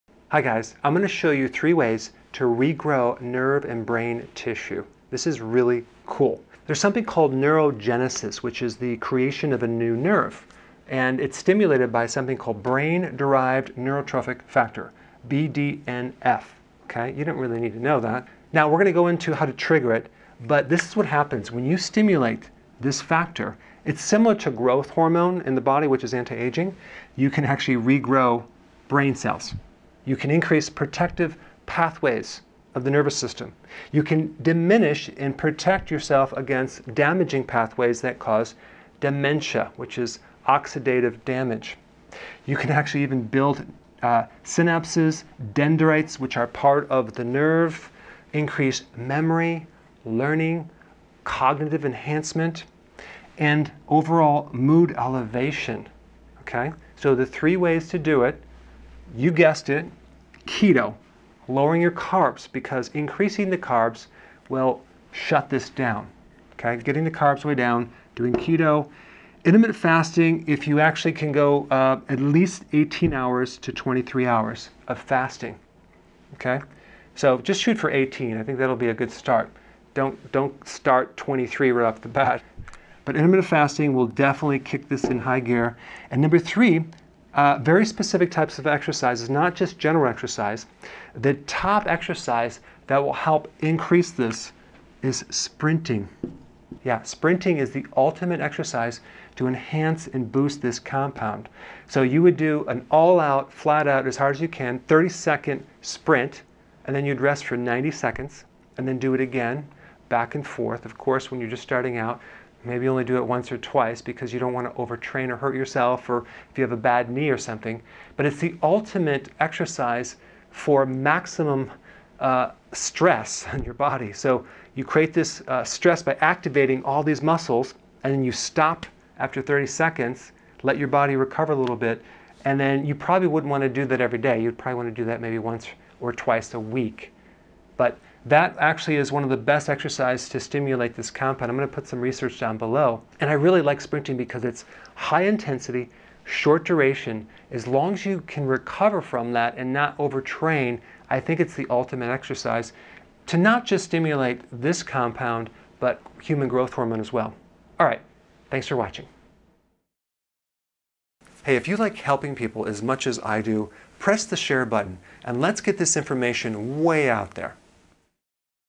Renew & Protect Your Brain Cells | Brain Derived Neurotrophic Factor – Dr. Berg
Discover 3 ways to regrow nerve and brain tissue to improve cognitive function and prevent degenerative diseases like dementia.

0:00 Introduction: What is neurogenesis?
0:18 Brain-derived neurotrophic factor (BDNF) and neurogenesis
0:57 3 ways to regrow nerve and brain tissue
2:38 Thanks for watching!

In this video, we’re going to talk about 3 ways that you can regrow brain cells and nerve tissue. Neurogenesis is the creation of new nerve cells.  This process is stimulated by brain-derived neurotrophic factor (BDNF), which is similar to growth hormone.

By stimulating neurogenesis you can experience the following benefits:
•Regrow brain cells
•Increase protective pathways of the nervous system
•Diminish and protect yourself from oxidative damage that causes dementia
•Rebuild synapses and dendrites
•Improved memory
•Cognitive enhancement
•Mood elevation

Here are the 3 ways to regrow brain cells and nerve tissue:
1. Keto
2. Intermittent fasting (18-23 hours per day)
3. Specific types of exercise (Sprinting and HIIT)

I hope this explains how to regrow brain cells and nerve tissue with keto and intermittent fasting.